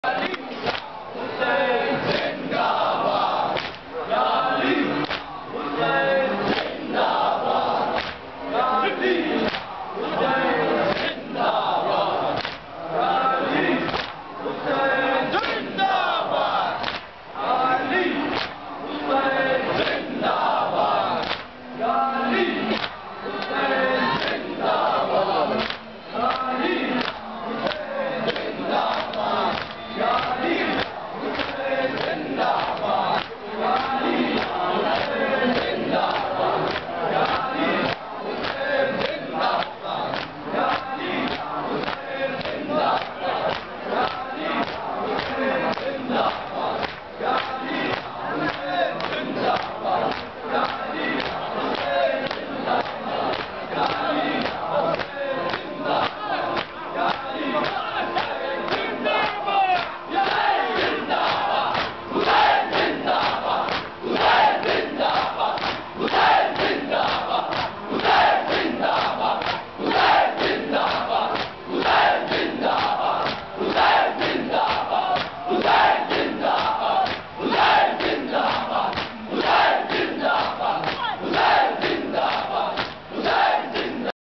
Jaunpur 18 SAFAR Matam at Islam Chowk, Jaunpur
Anjuman Sajjadia Mufti Mohalla, Jaunpur doing matam at Islam Chowk,Jaunpur on 18 safar 2011.

You can explore more videos of nauha, majalis,matam, marsia and other shia articles at our main